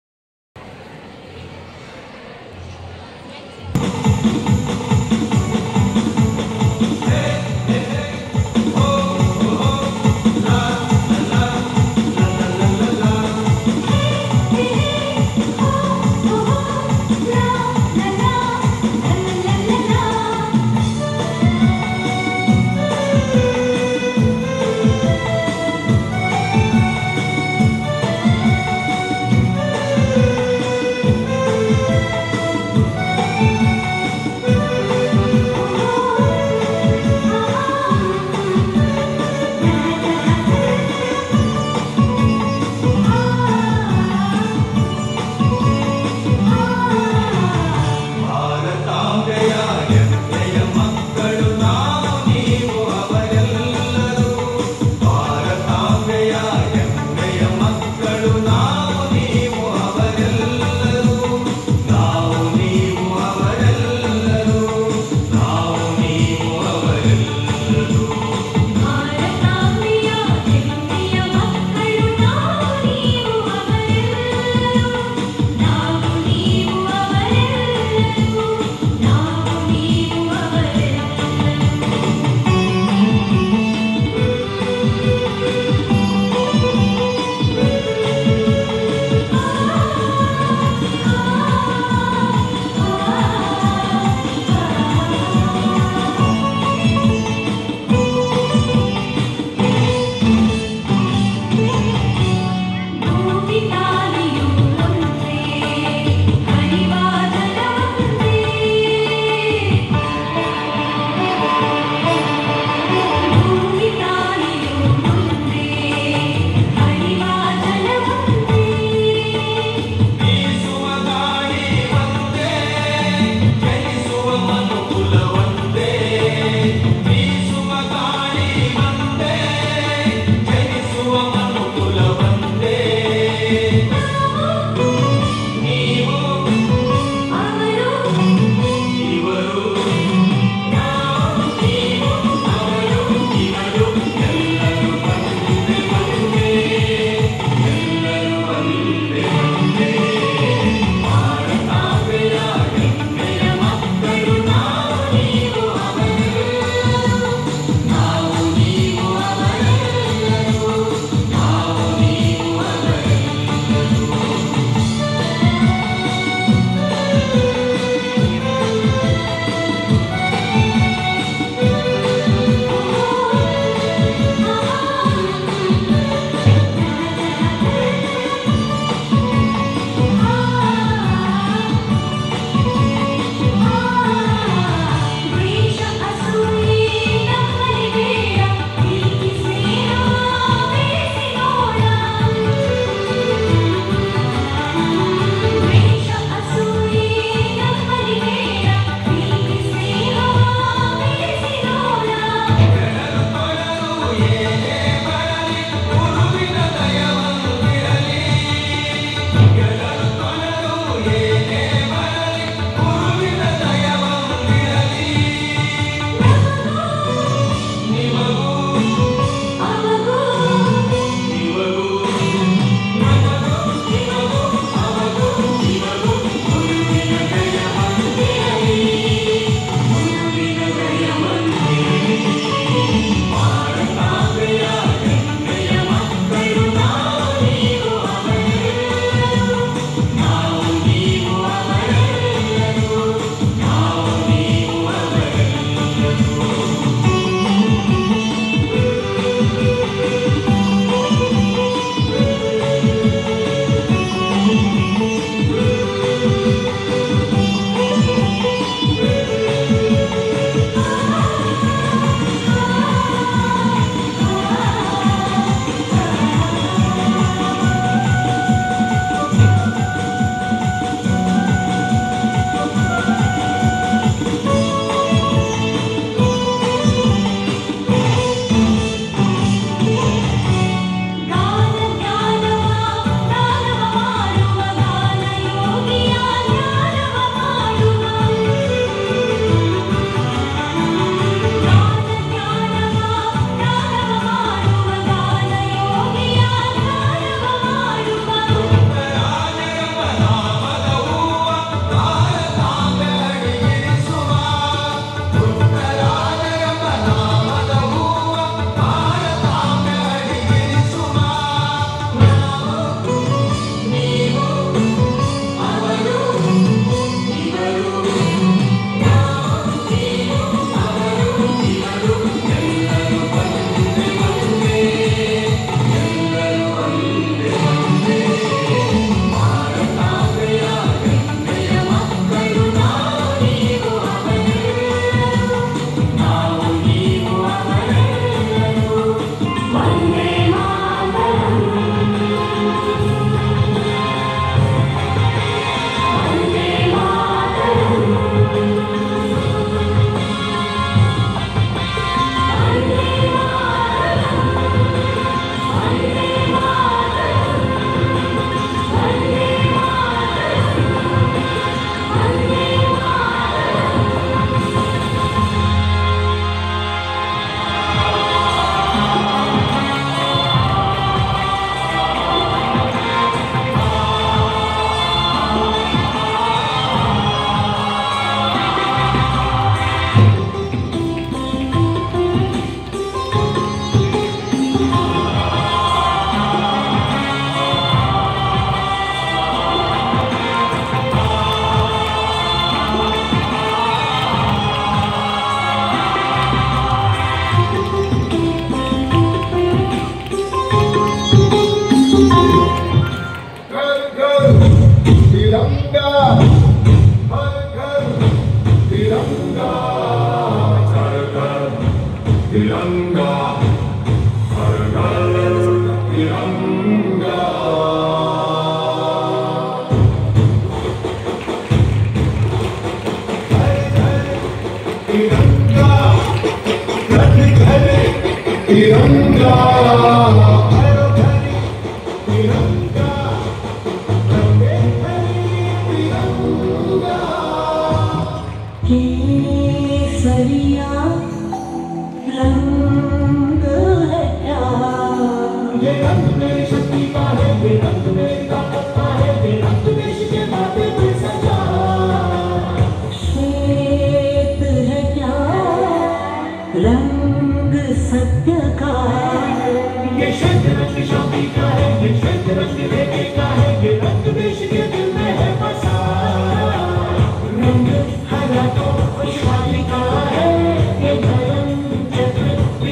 Vijaya Vittala Vidyashala - 76th Independence day Celebrations - 2022
Saree Dance by group of high school girls.

Video Credits: Sri. Raghavendra K V, P E teacher, VVVS